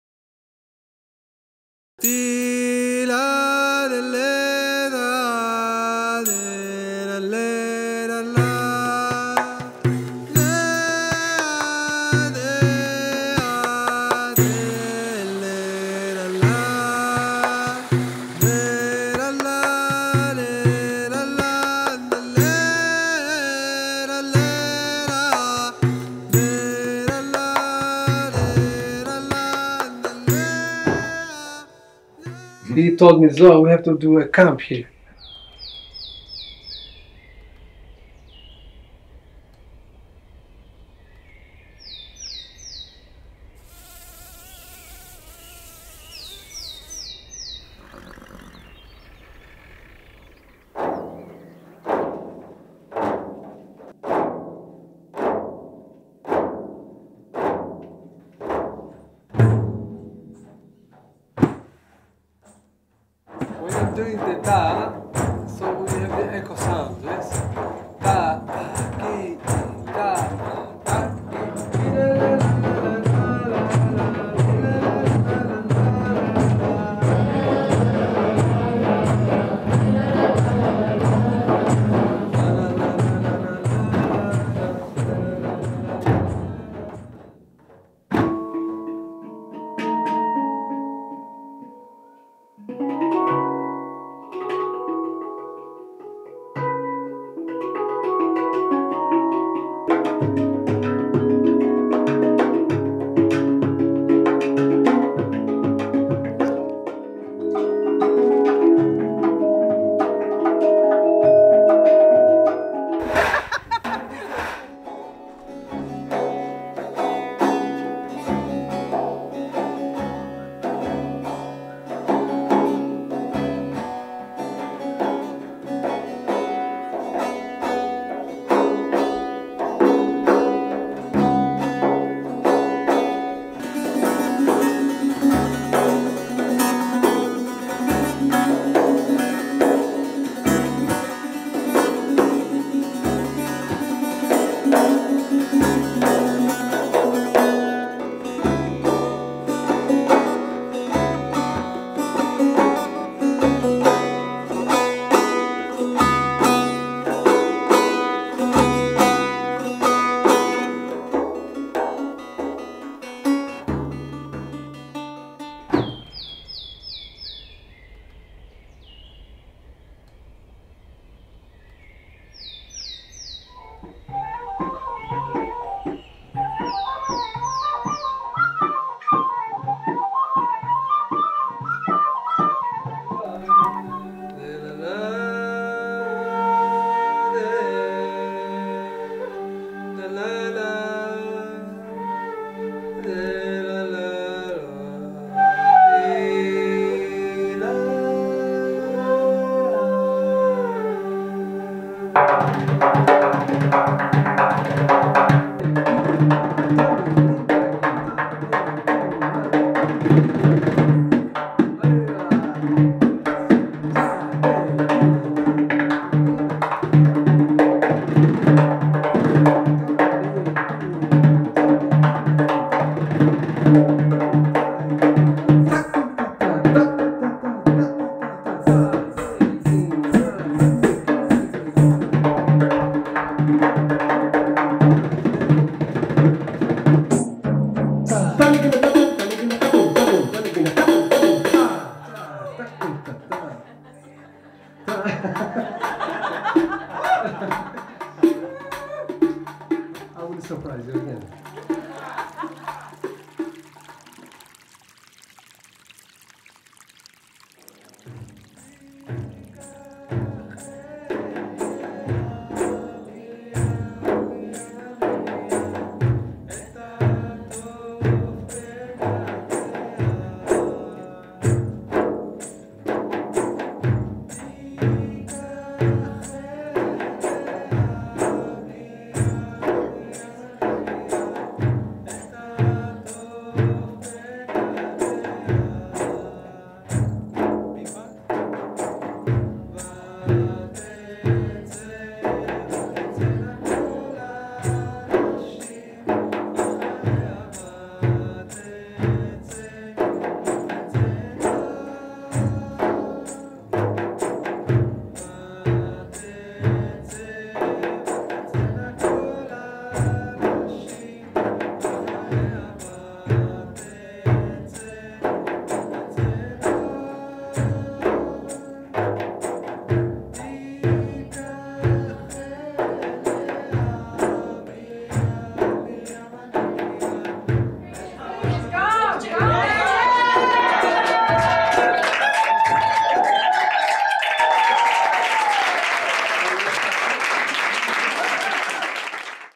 Zohar Fresco Framedrums Camp 2024
I had the pleasure of listening to unique sounds and music. In one day, this story was created.

Over forty people from all over the world met between 8th and 18th August 2024 in Silpia Duża, to celebrate life and the gift of the frame drum under the guidance of Zohar Fresco.
The camp was organized by Jacek Żelazek and Joanna Zakrzewska-Żelazek from Living in the Rhythm Fundation and ZelazekDrums company
Next framedrums camp with Zohar Fresco is on 8-17 August 2025